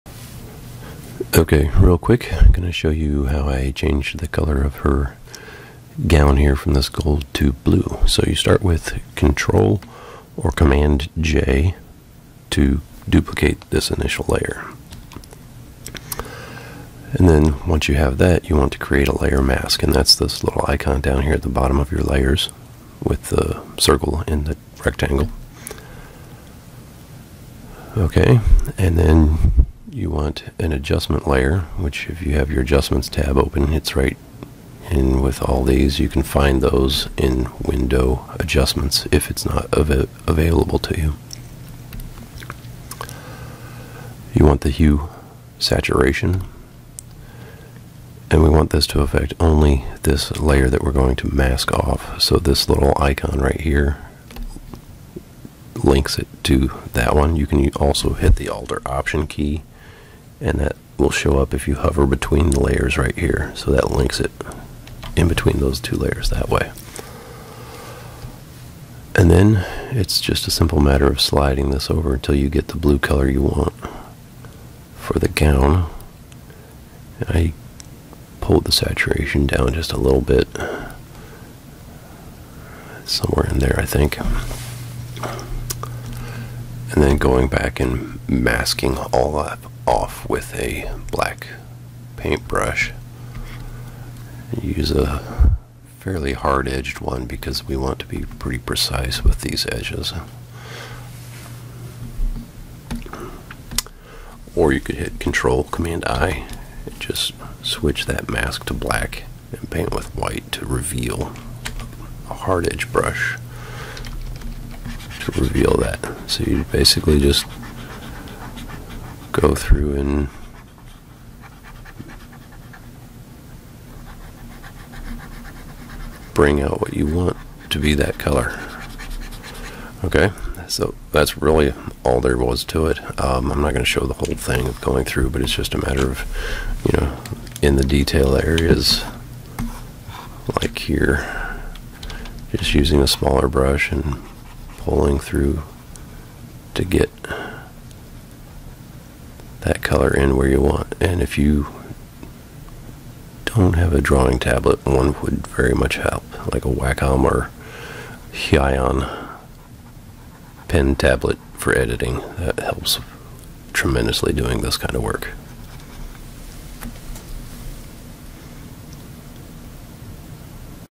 Photoshop - Gown color change
How to change the color of a graduation gown